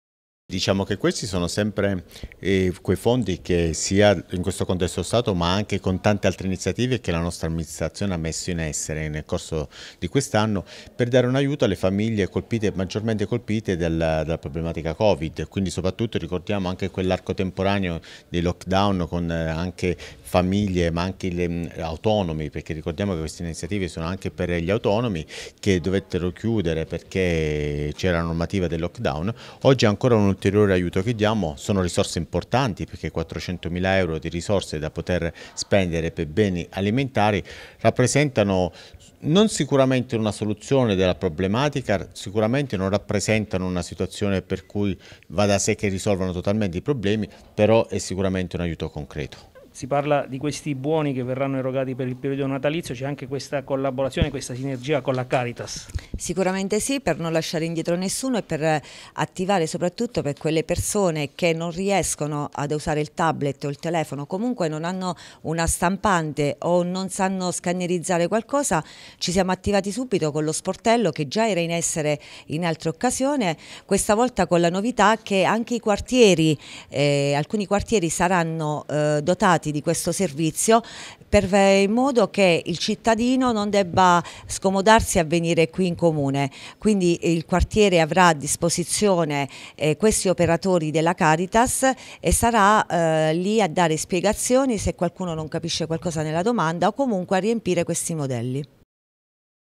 Pescara. Comune distribuisce i buoni spesa
L’Amministrazione Comunale di Pescara in vista delle festività natalizie, erogherà i buoni spesa destinati alle famiglie in difficoltà, fortemente provate dalla pandemia. A disposizione 400mila euro